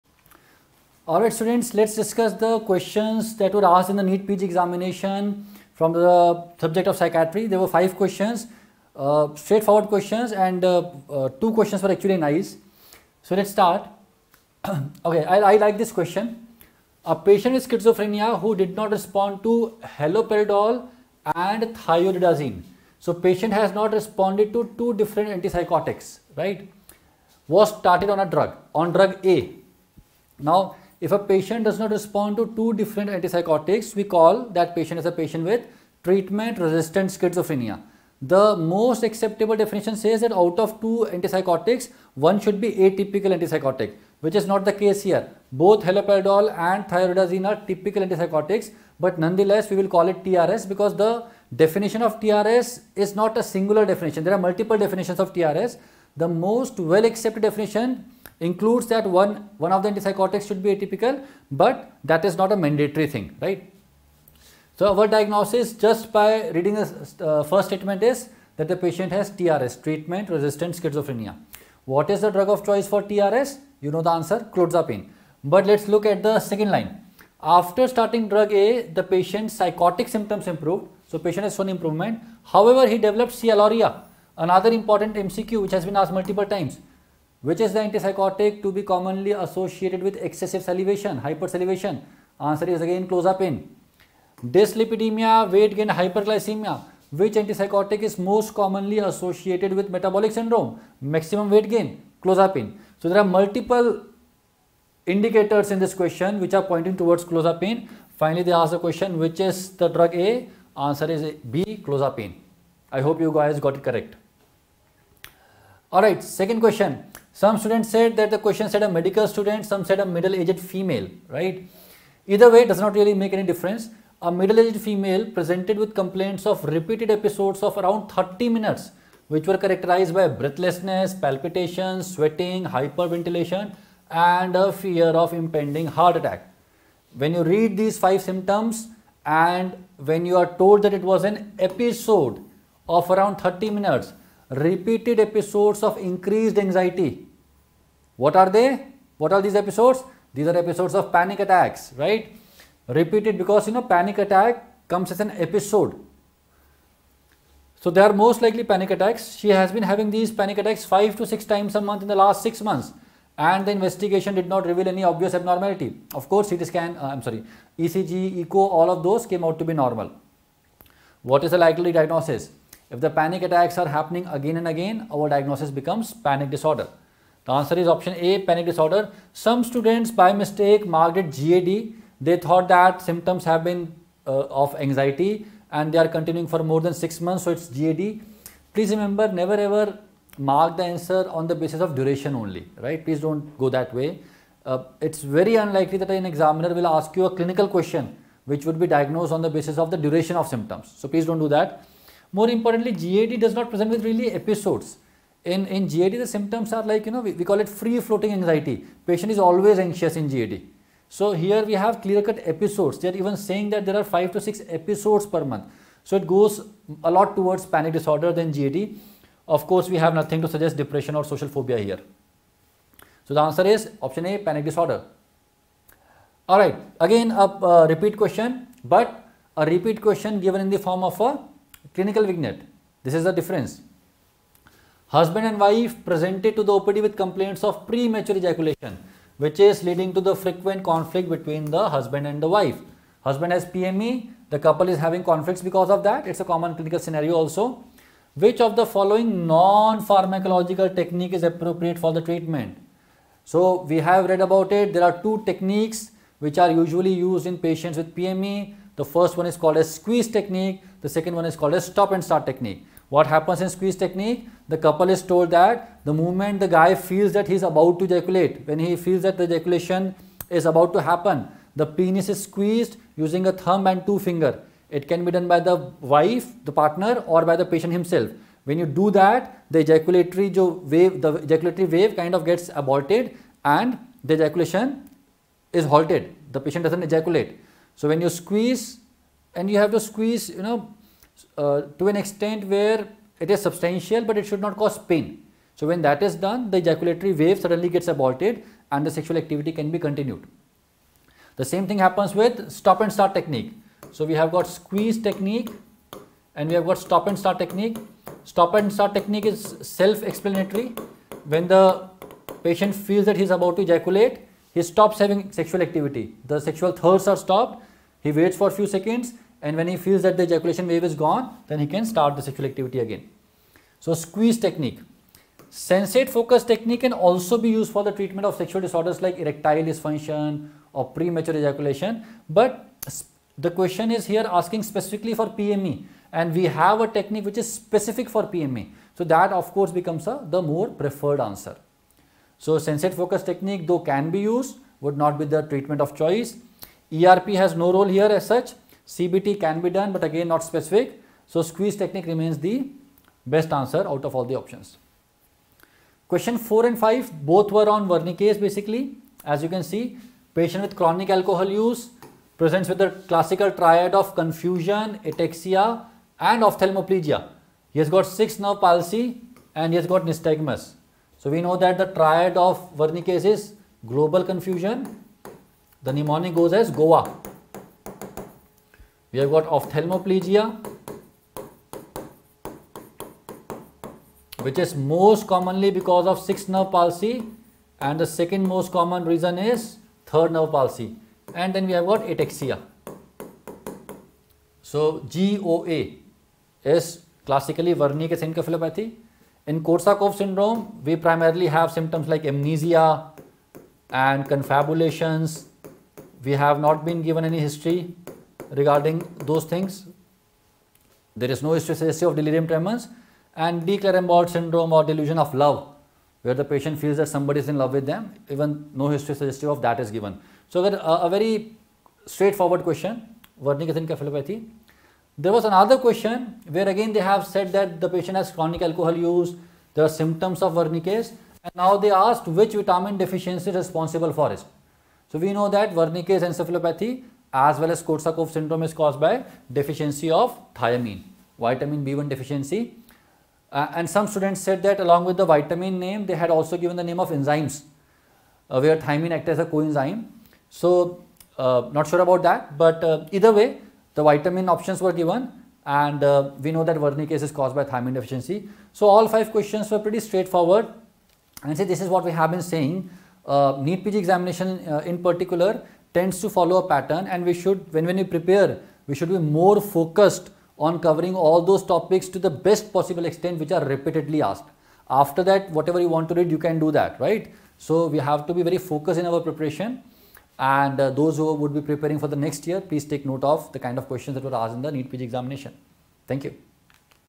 NEET PG 2023. Psychiatry Recall by Dr. Praveen Tripathi
NEET PG 2023 recall based Psychiatry questions discussed by Dr. Praveen Tripathi